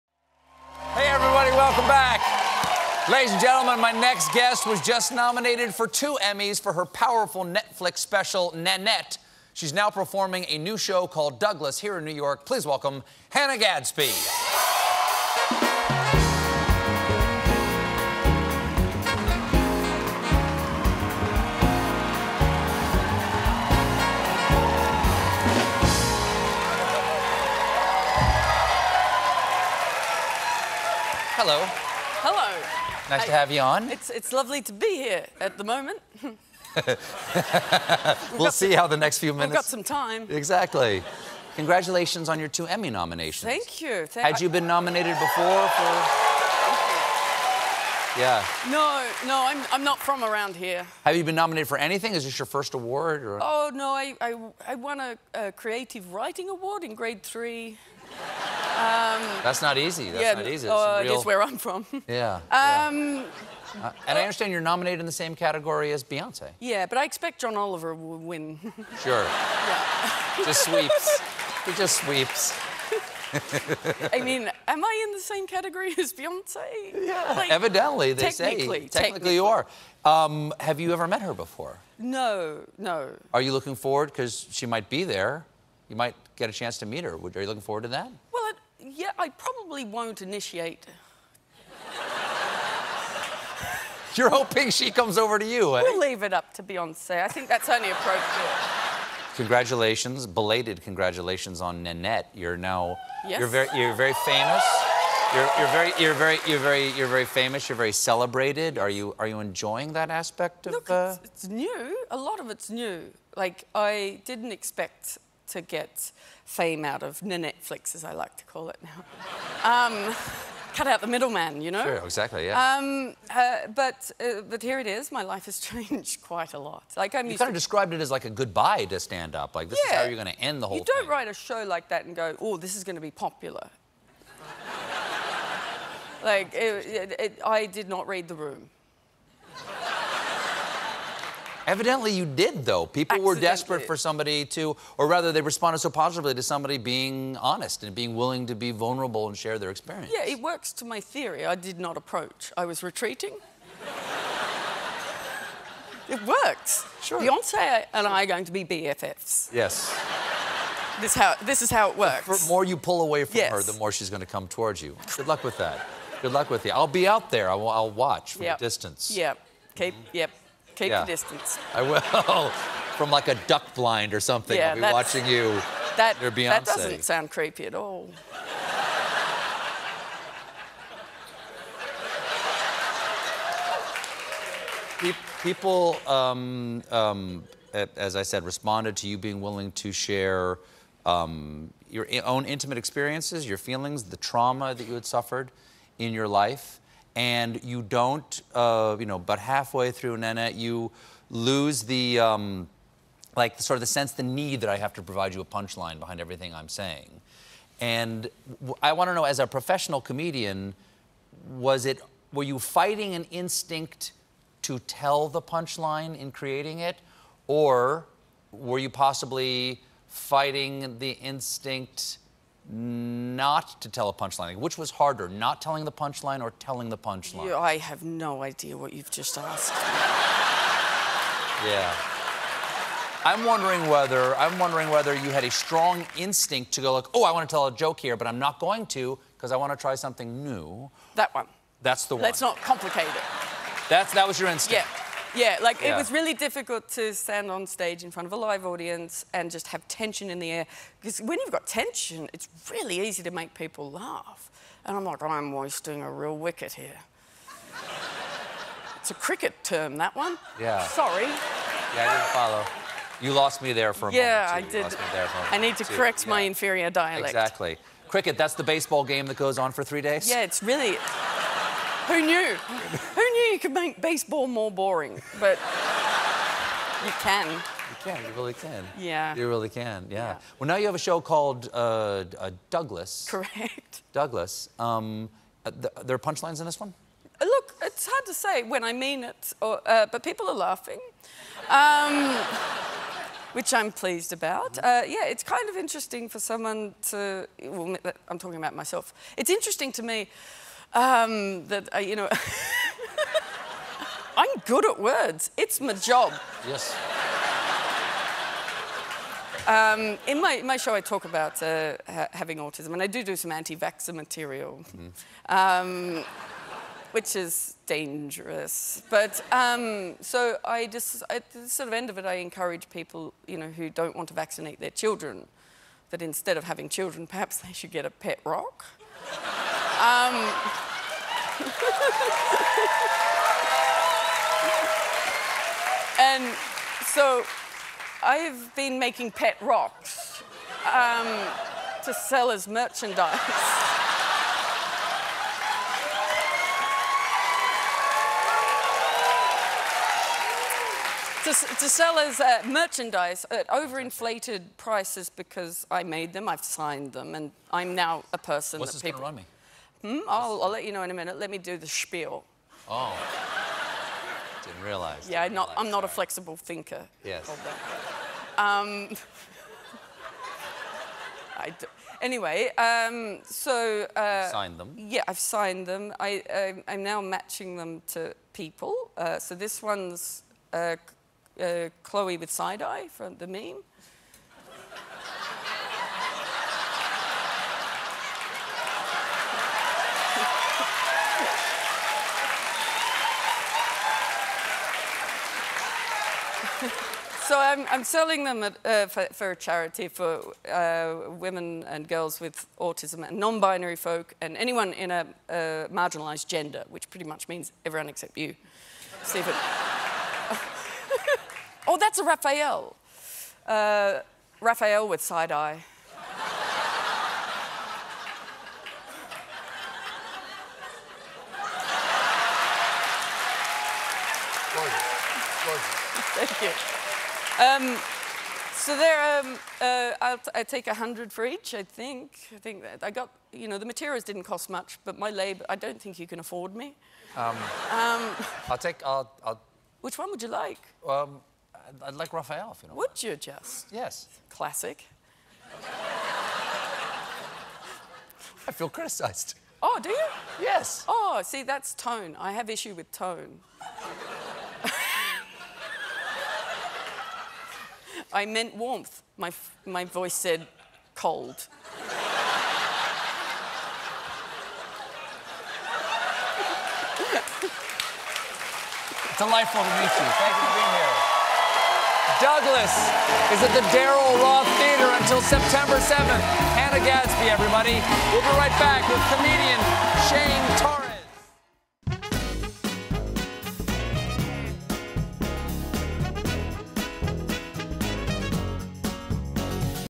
Hannah Gadsby Doesn't Plan On Initiating Conversation With Beyoncé
Comedian Hannah Gadsby was nominated for an Emmy in the same category as Beyoncé, but don't expect the two to meet in person.